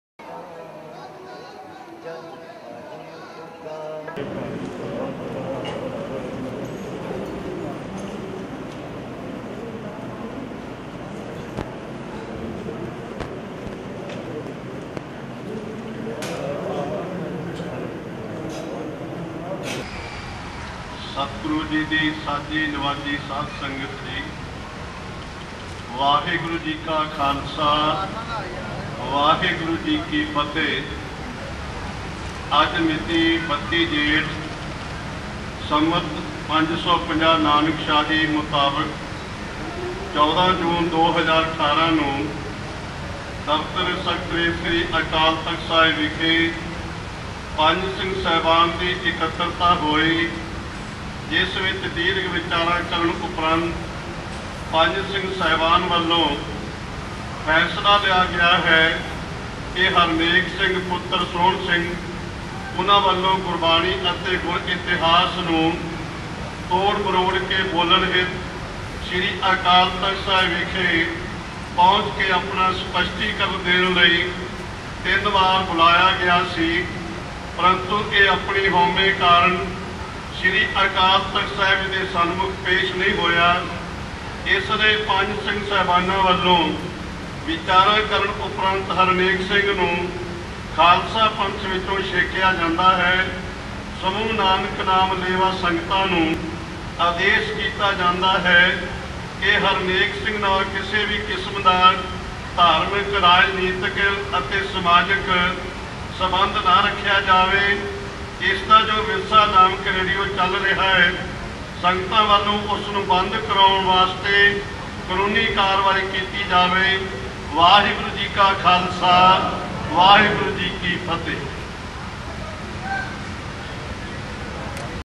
Akal Takht excommunicates New Zealand-based Sikh radio host for derogatory comments
Akal Takht, the supreme temporal seat of the Sikhs, on Thursday excommunicated New Zealand-based Radio Virsa host Harnek Singh Neki over derogatory comments on Sikhs Gurus and present leaders of the community.

Neki was summoned to the Akal Takht twice by jathedar Giani Gurbachan Singh but he failed to make an appearance, prompting the five Sikh high priests to decide on his excommunication.

Takht Kesgarh Sahib jathedar Giani Raghbir Singh, Takht Patna Sahib jathedar Giani Iqbal Singh, Takht Damdama Sahib jathedar Giani Harpreet Singh and Akal Takht head granthi Giani Malkeet Singh attended Thursday’s meeting during which the resolution to excommunicate Neki was passed.

The charges against the radio host relate to twisting Sikh history, concepts, and religious texts.